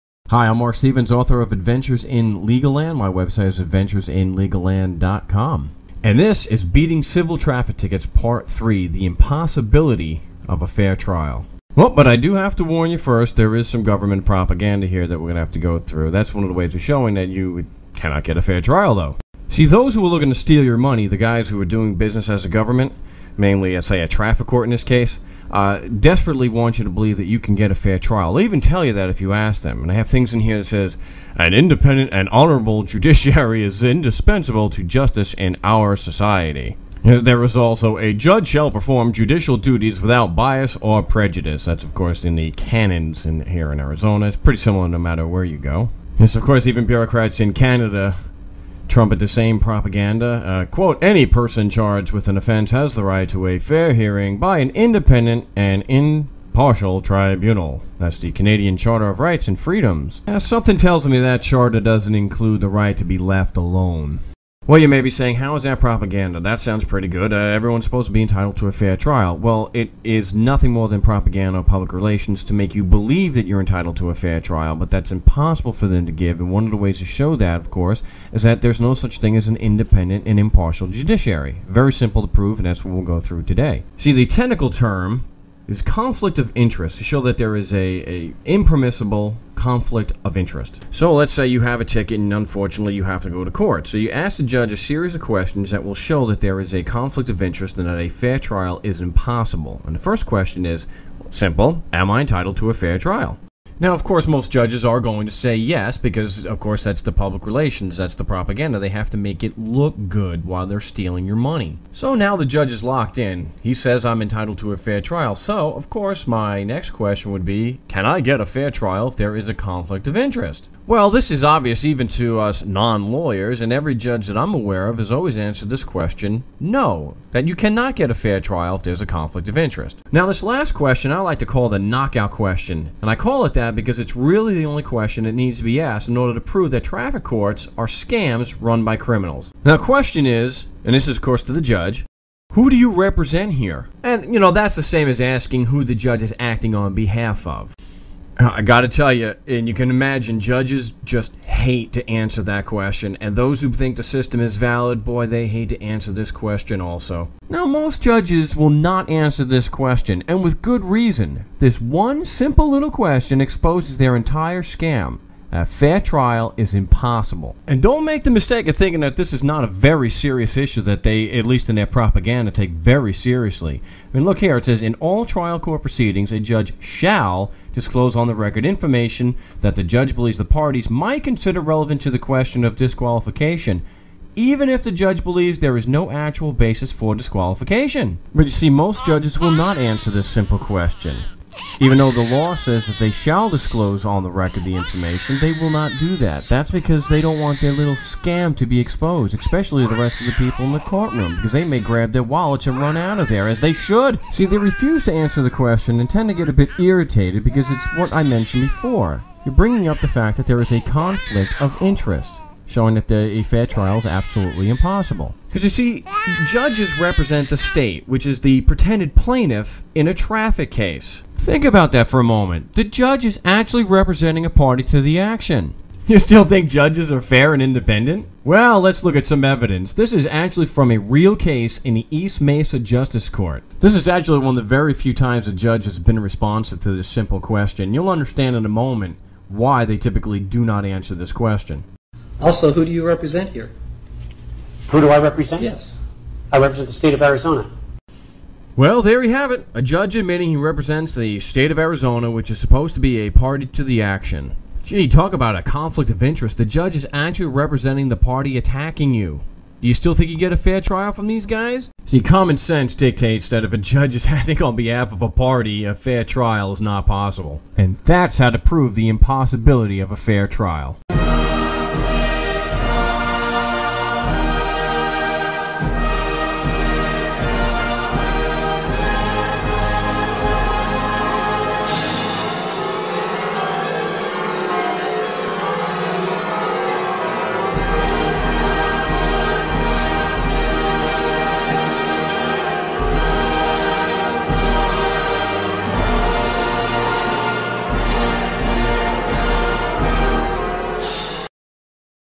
Beating Civil Traffic Tickets - Part 3 Fair Trial Impossible - [KOPIMI] 03/14/13
I give the credit to Calv DeWitt. Patriot of the Republic 1, maf1989b (Mike Fulmer).

February 5, 2008 - "Beating Civil Traffic Tickets - Part 3 Fair Trial Impossible"

KOPIMI